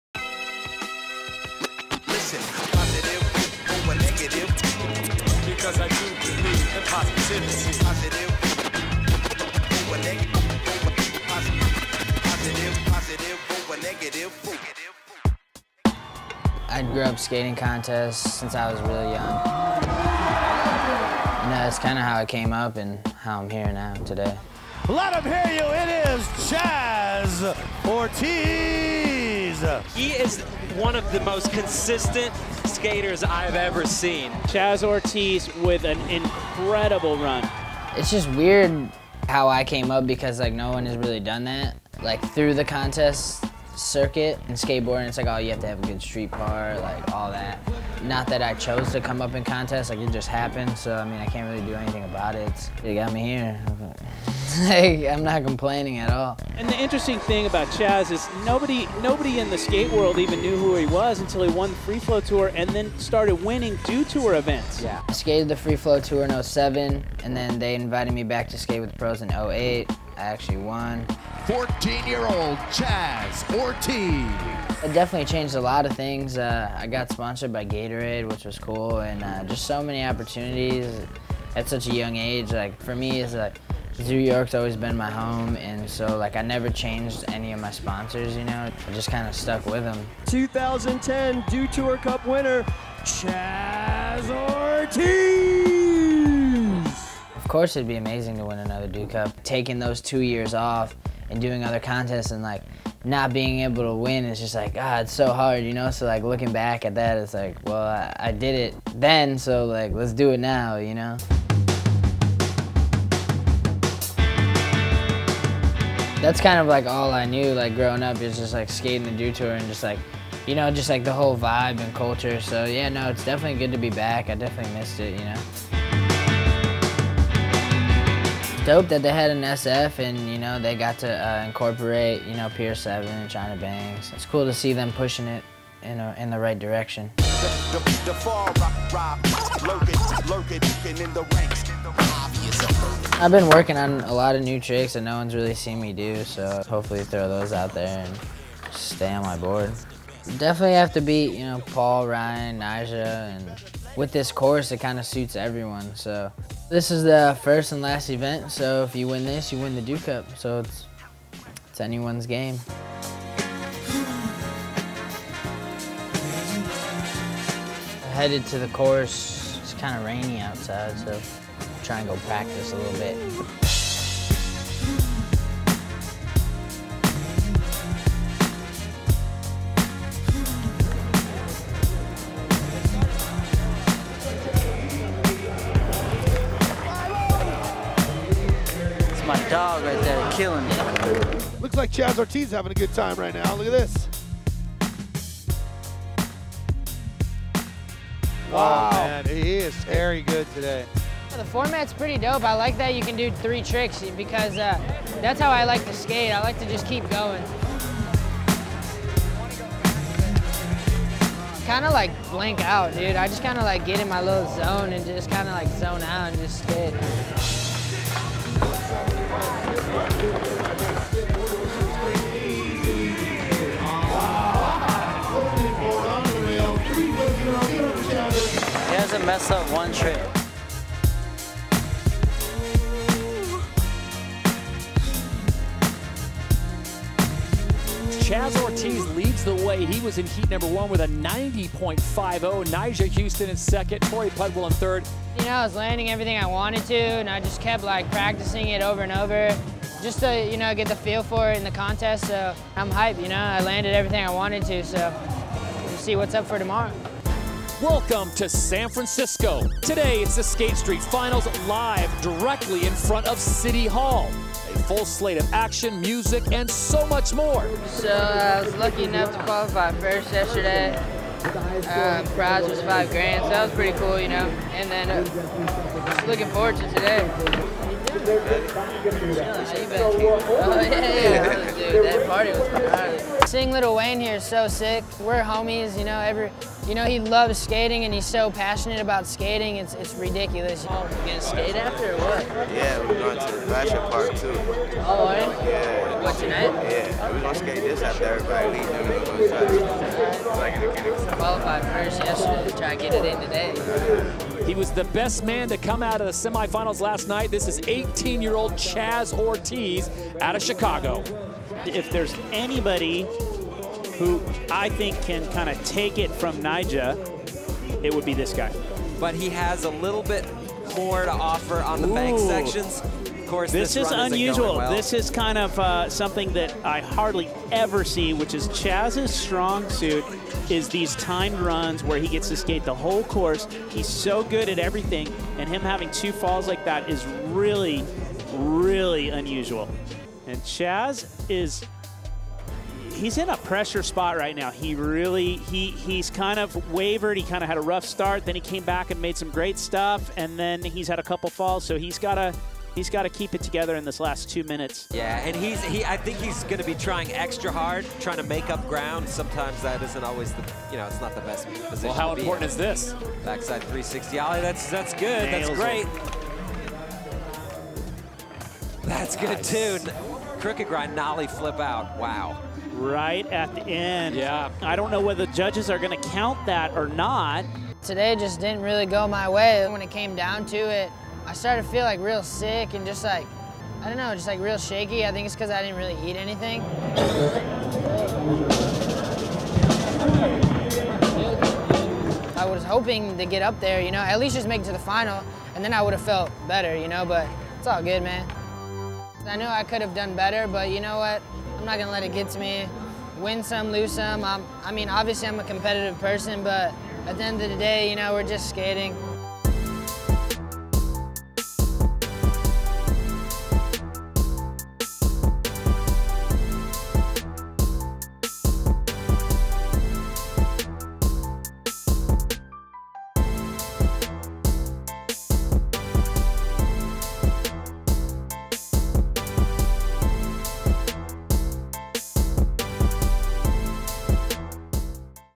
Chaz Ortiz Skating Dew Tour's San Francisco Street Plaza | Inside Alli Sports
Follow Chicago native Chaz Ortiz through his first Dew Tour event since winning the 2010 Dew Cup two years ago. Chaz has high hopes for this event and is excited to skate against contest favorites Paul Rodriguez, Torey Pudwill and Nyjah Huston. Ride around the street course with Chaz Ortiz and hear his thoughts on how everything went down as he takes you Inside Skate at the Dew Tour.

Got an opinion on one of the topics in the show? Want to let us know how we're doing? Hit us up on twitter at @AlliSports.